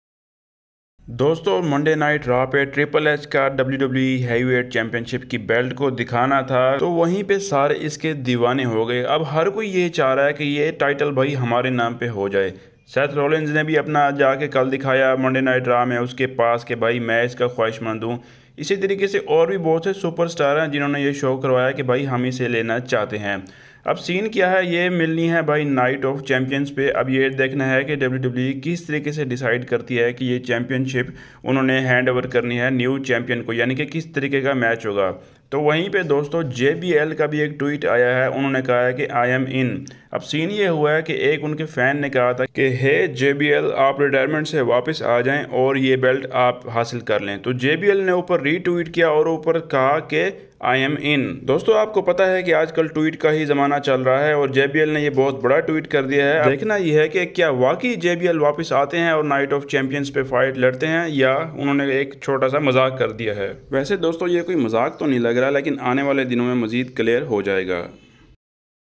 WWE Legend says he's ready to come out of retirement for the new World Heavyweight Championship
On Twitter, a fan asked JBL to come out of retirement for the new World Heavyweight Championship, and he said he is in.